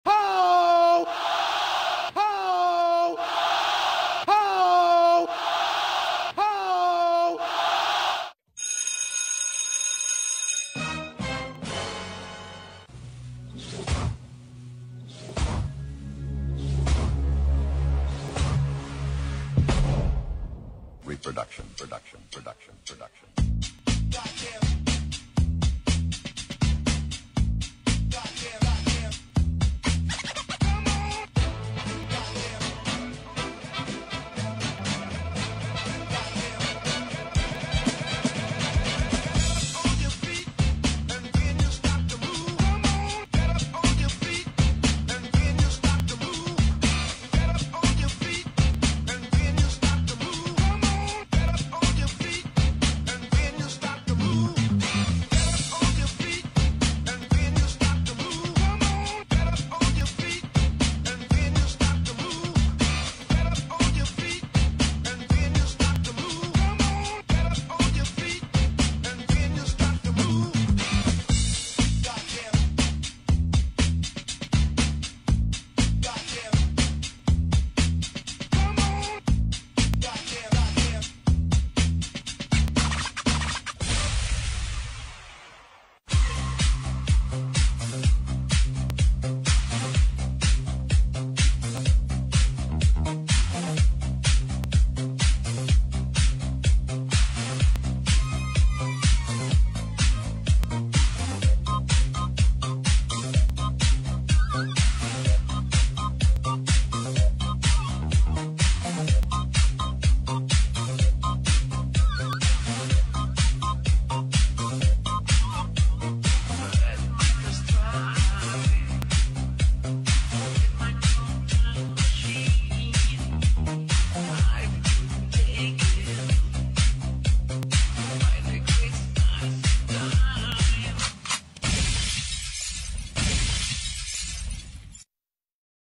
Udo Cyprus OPEN 2023 - U14 - SCOOBY LOCK - 1ST PLACE
Udo Cyprus OPEN STREETDANCE CHAMPIONSHIPS 2023
U14 DIVISION
QUALIFIED FOR WORLD & European UDO STREETDANCE CHAMPIONSHIPS

Videography: Suzana Phialas